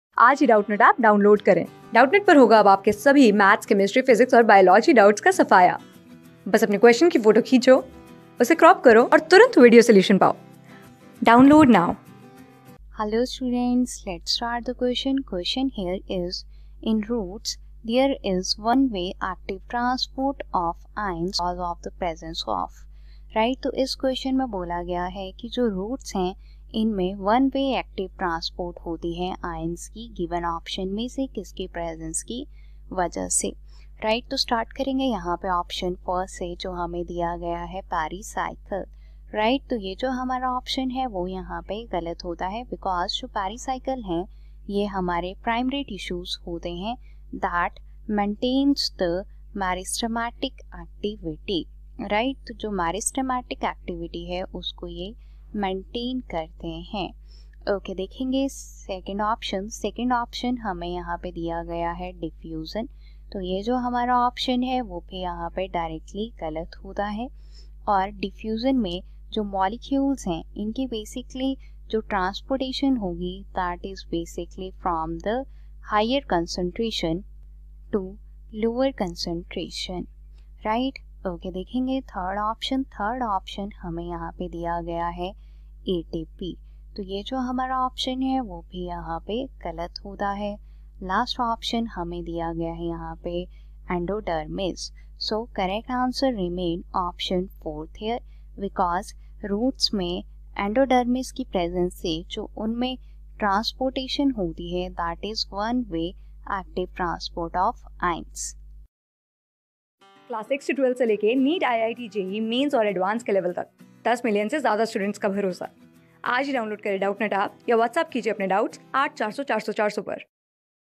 In roots, there is one way active transport of ions because of presence of :- | CLASS 12 | NEET-...
In roots, there is one way active transport of ions because of presence of :-
Class: 12
Subject: BIOLOGY
Chapter: NEET-UG DRILL TEST 14
Board:NEET

👉 Have Any Query? Ask Us.